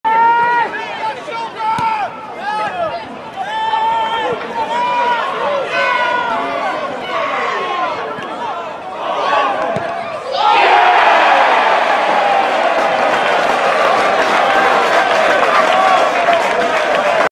Zane Banton vs East Thurrock United
Zane Banton's goal in the National League South fixture vs East Thurrock United